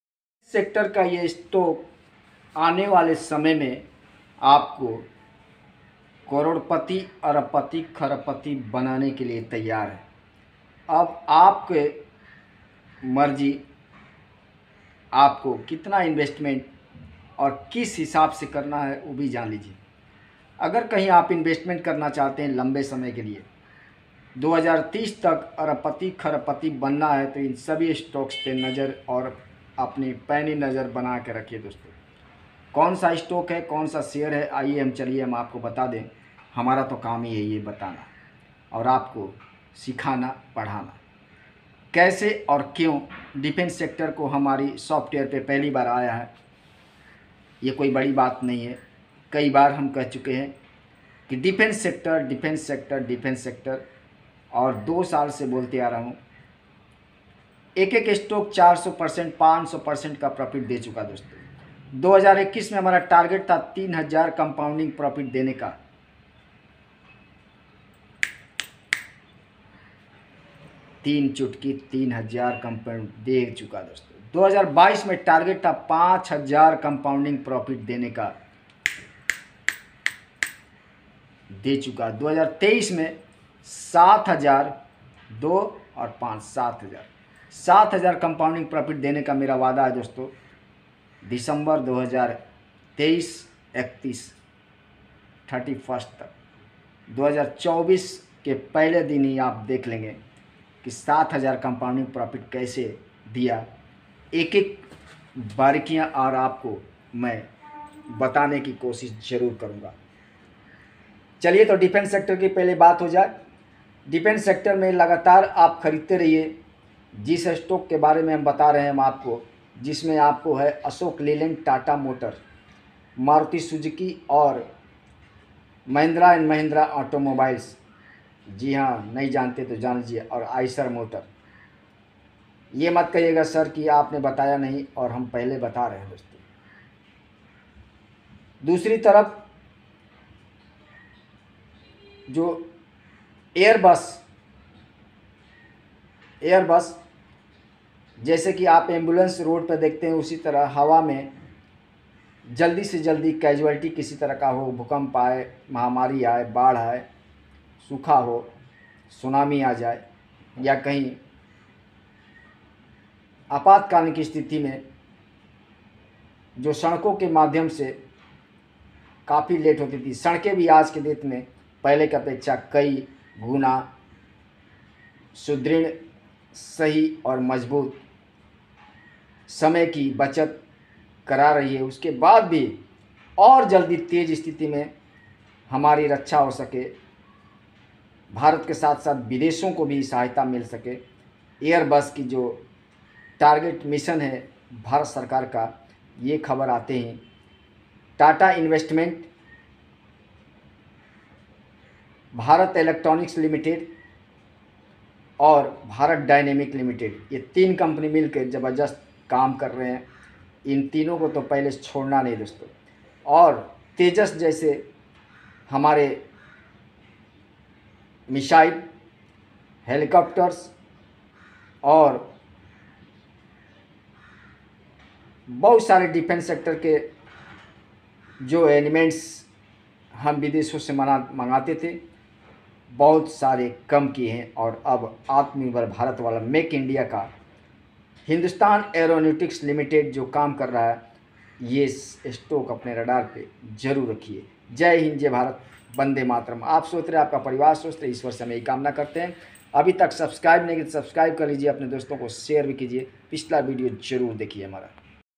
Deffence Sector का कौन सा स्टॉक है जो रातों रात करोड़पति से अरबपति बना देगा!
अगर आप स्टॉक मार्केट में पैसा बनाने आए हैं,तो ये जान लीजिए की  कैसे लोग अरबपति बनते हैं!